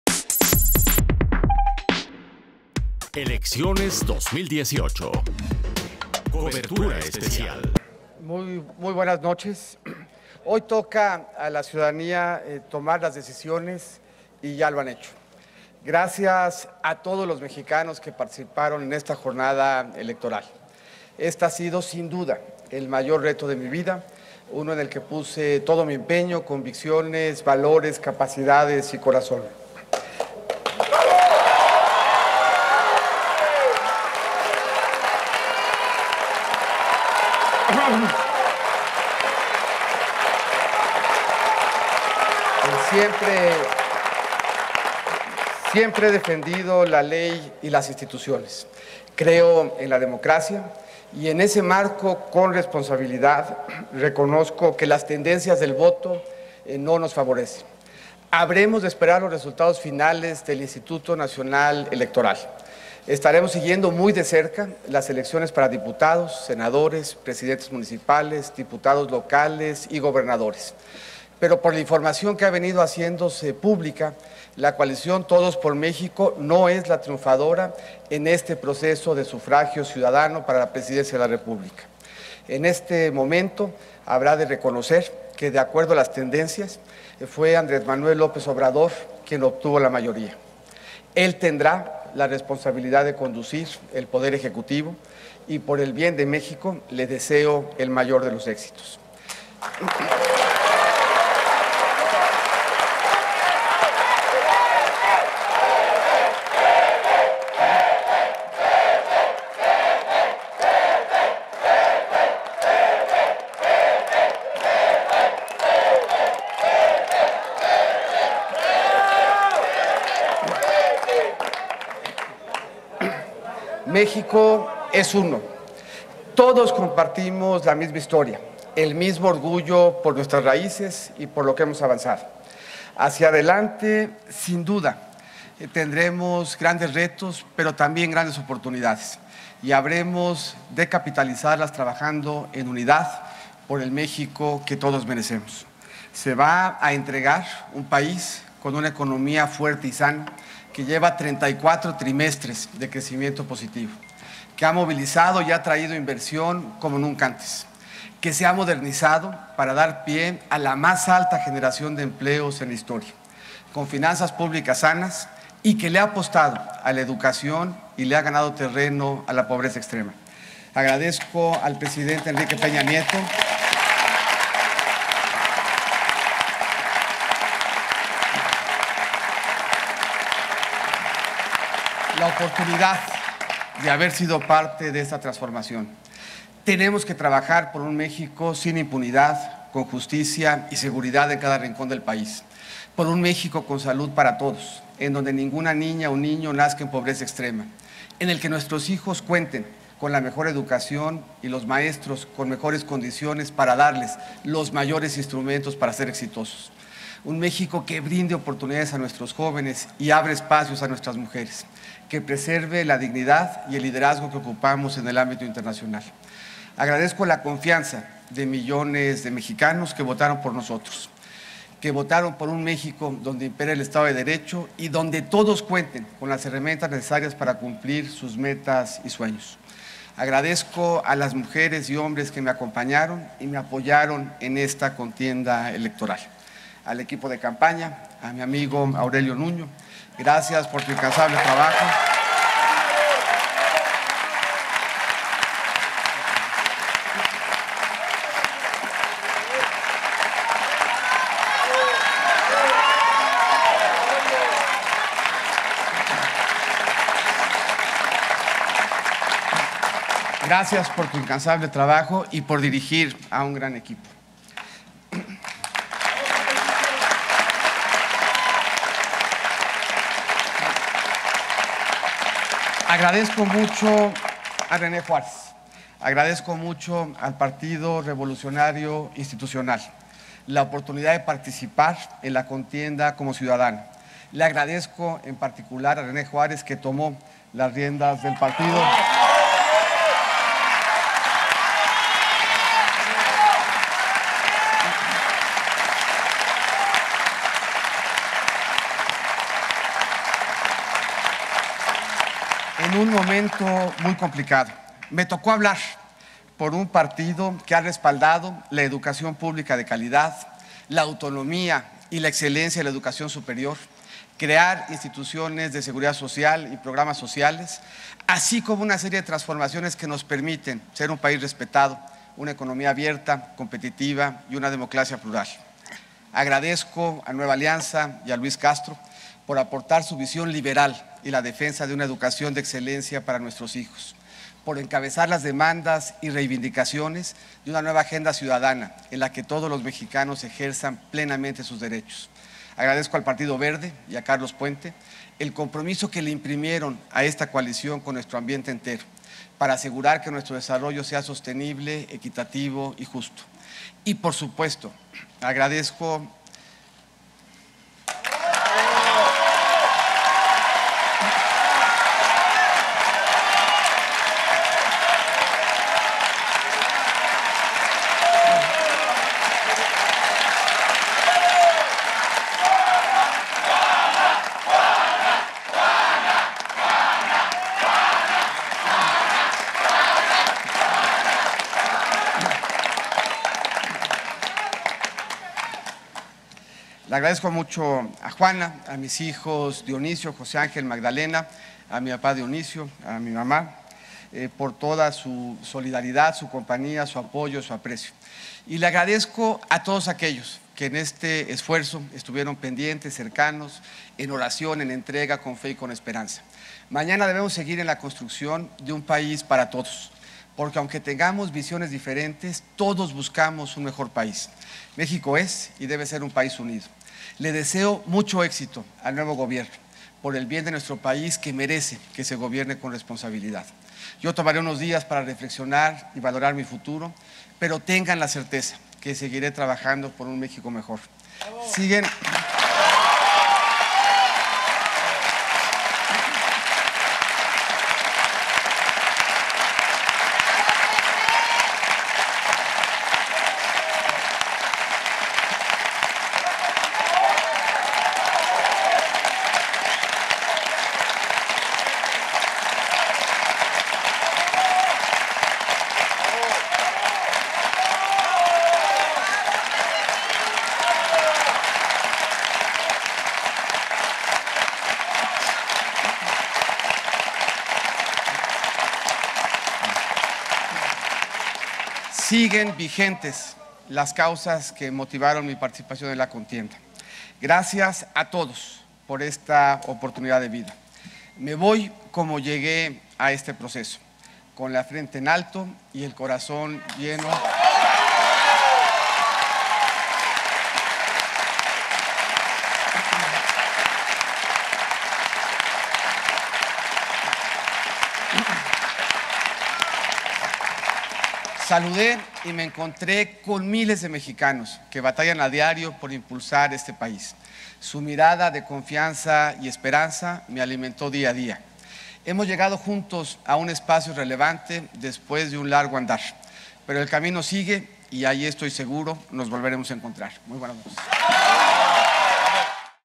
Meade reconoce que tendencias favorecen a López Obrador
El candidato presidencial de la coalición Todos por México, José Antonio Meade, admitió que las tendencias electorales no le favorecen y reconoció el triunfo de Andrés Manuel López Obrador, abanderado de la alianza Juntos Haremos Historia.